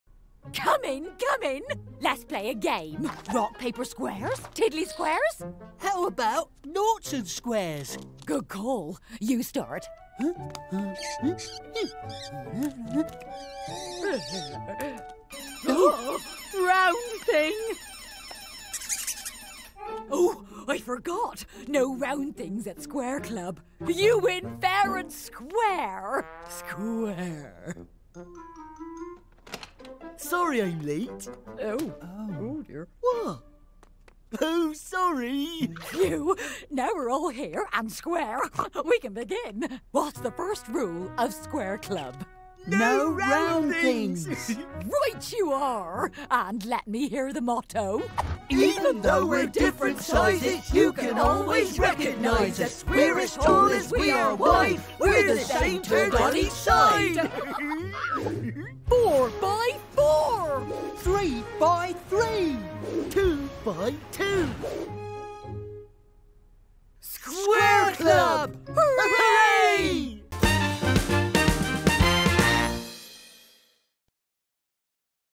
Square Numbers! 🟩 | Learn to Count
Learn about the square numbers in the square club with 4, 9 and 16!